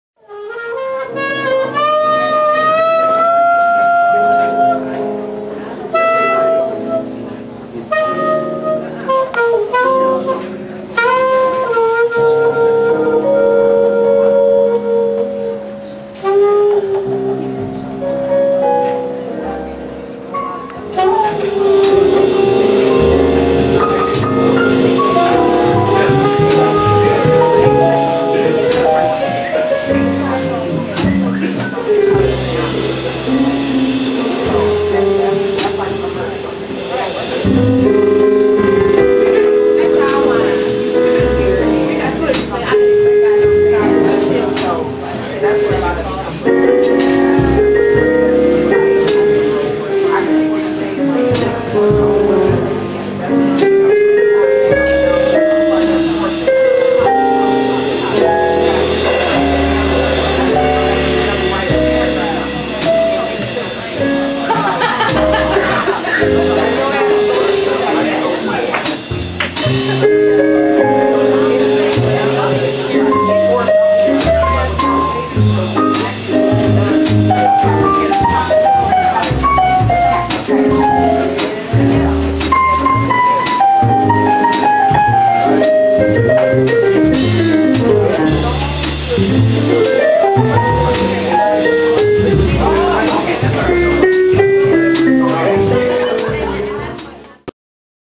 Dennis Luxion on keys
This LIVE MUSIC event was Ernest Dawkins Jazz Jam on Sunday, December 29, 2013, from 9 pm - 12 am. This jam session is every Sunday all year long! Performing on this night were Ernest Dawkins on sax, vocals and percussion, Dennis Luxion on keys, Darius Savage on bass and Isaiah Spencer on drums.

The LIVE MUSIC venue is Norman's Bistro, 1001 E 43rd St. (43rd and Ellis), Chicago, IL 60653, (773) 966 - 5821.

, has a set of photos from this special night - - check them out!